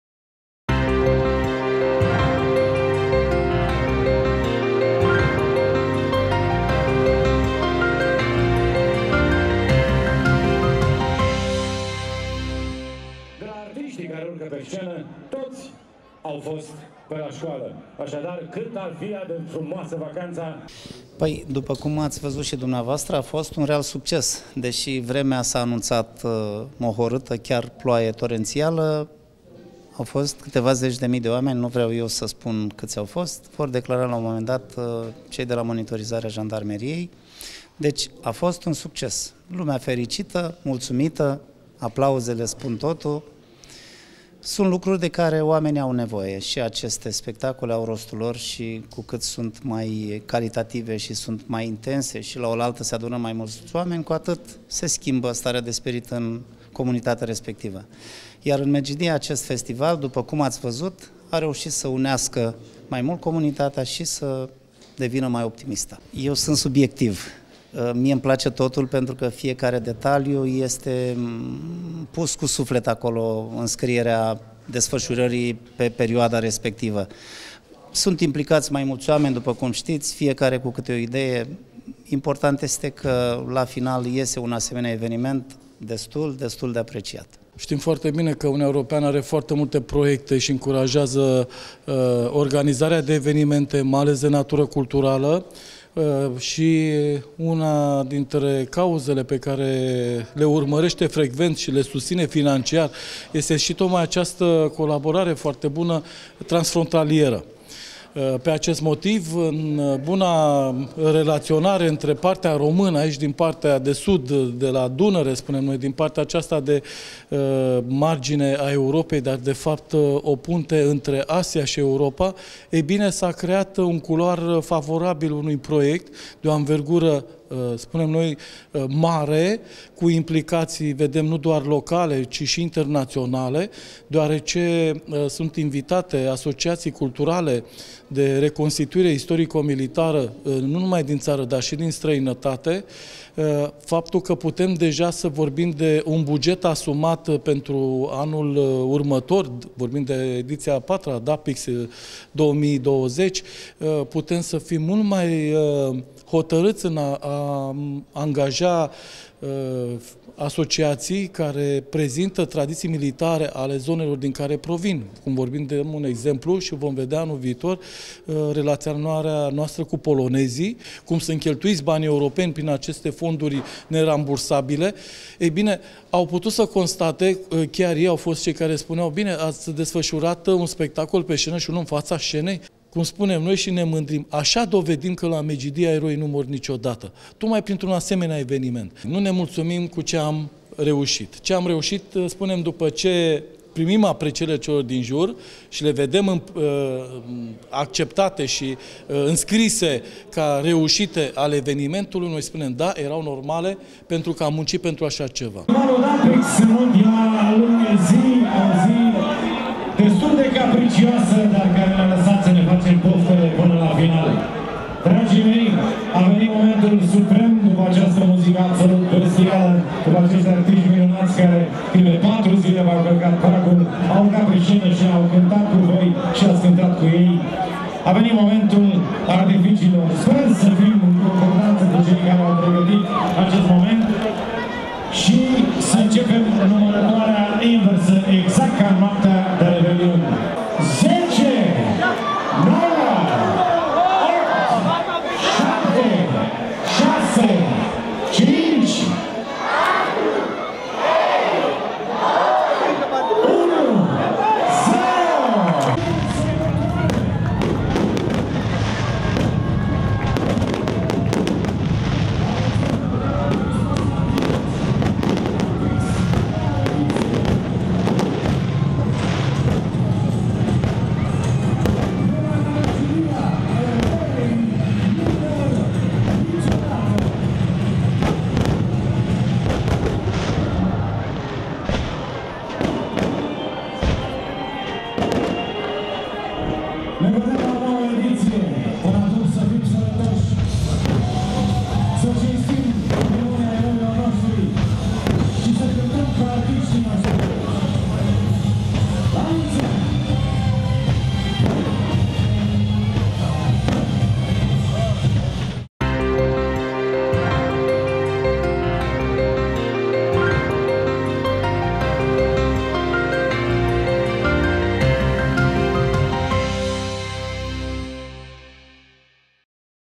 CTnews.ro |Zeci de mii de persoane au participat la Festivalul Dapyx de la Medgidia
Zeci de mii de persoane au fost prezente la cea de-a treia ediție a Festivalului Dapyx de la Medgidia, oamenii urmărind atât atelierele de reconstituire istorică, dar și concerte susținute de artiști îndrăgiți. Un reportaj realizat de jurnaliștii Constanța NEWS